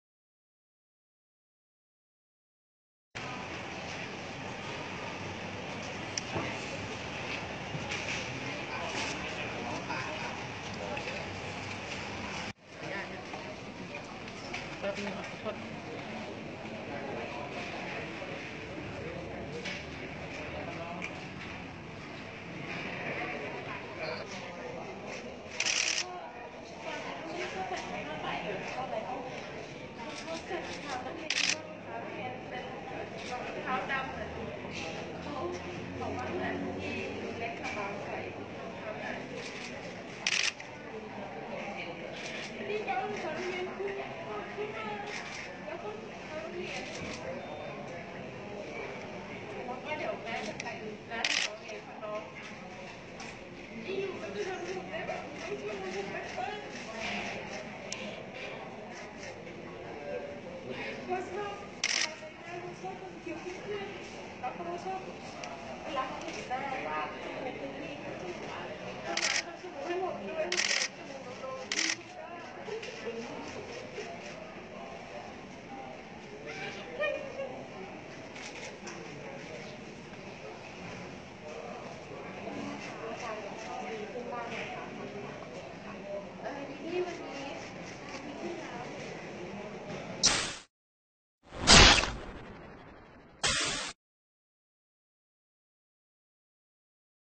แม่ร่ำไห้รับศพอเล็กซ์ l อีจัน Ejan
วันนี้ 4 พ.ค.2561 น.ส.รัตติกาล ประกายแก้ว แม่ของเด็กที่พลัดตกตึกทั้ง 4 คน จะเดินทางเข้ารับศพของ ด.ช.คนัธชา นิรันดร์ โบเรล น้องอเล็กซ์ ลูกชายคนเล็กที่โรงพยาบาล และจะนำศพไปบำเพ็ญกุศลที่วัดจันทวงศาราม (วัดกลาง) โดยจะมีพิธีรดน้ำศพช่วงเวลา 17.00 น. เเละตั้งใจจะเก็บศพน้องไว้ 100 วัน